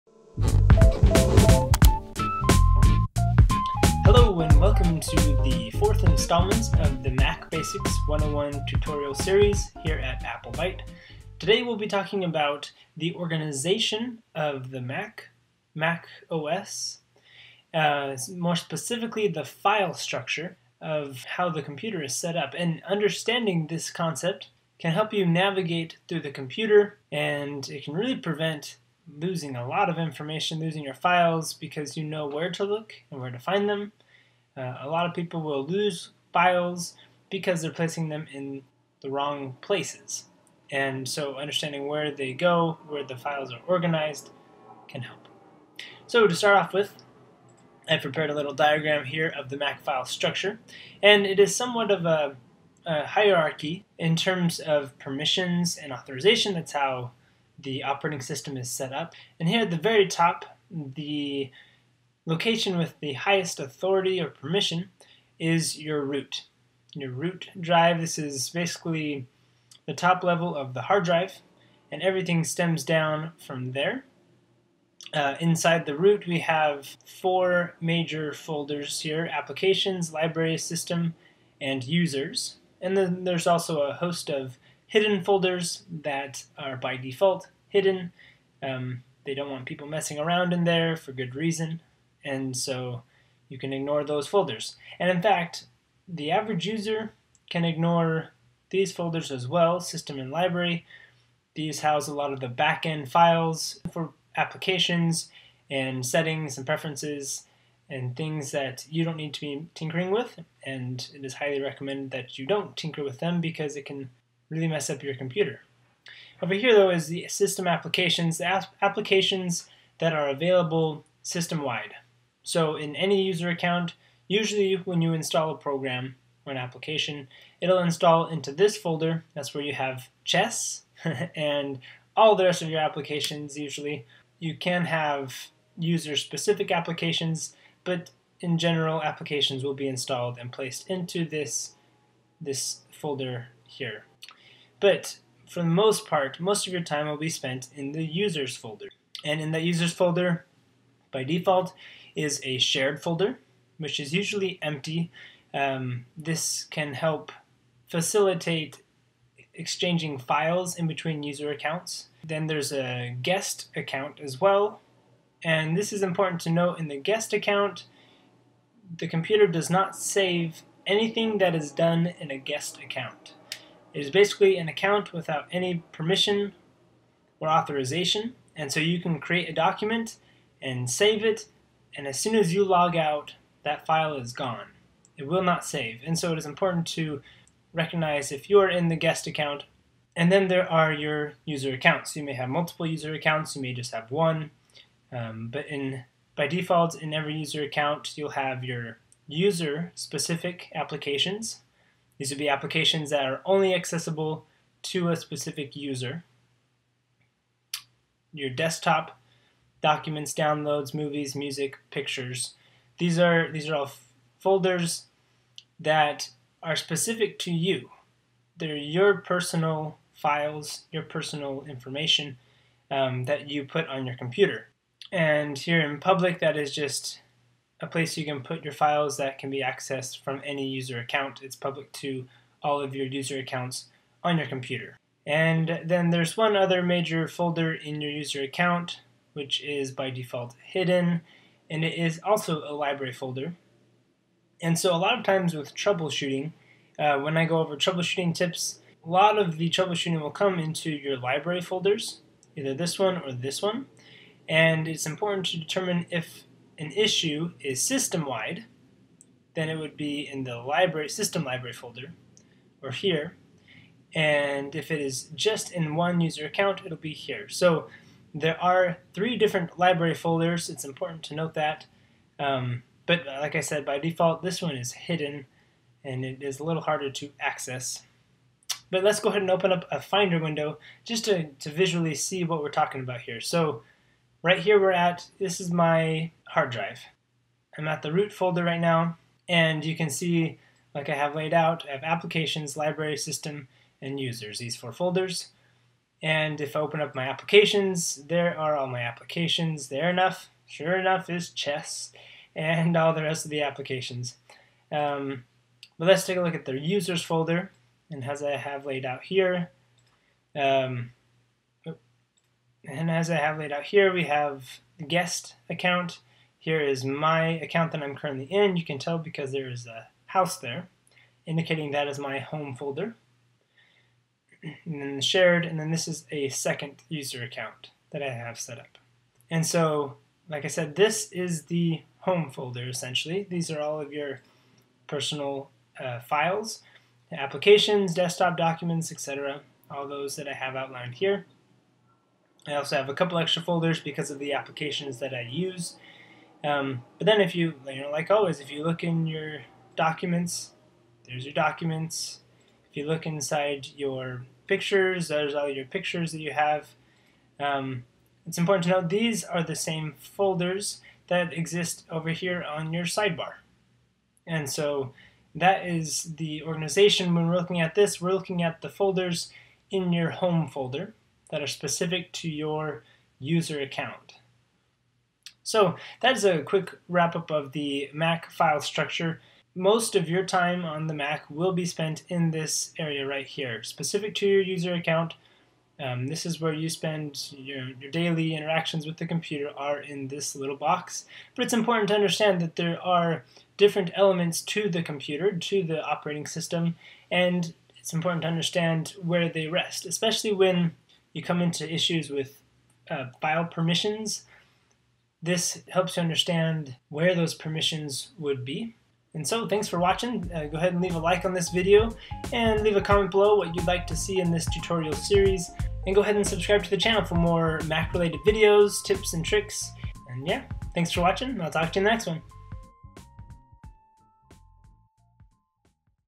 Mac Basics 101: File Structure
My complete course for learning the Mac is out on Udemy!

4th video in the Mac Basics 101 Tutorial series- we cover what the file structure is on Mac and answer questions like What is the root folder? Where are applications saved on my Mac? What is the Home folder or where is the Library folder on my Mac?

This tutorial series is for mac beginners and those who have recently purchased a new Mac or switched from Windows. If you're wondering where to begin with a new Mac, this is the place.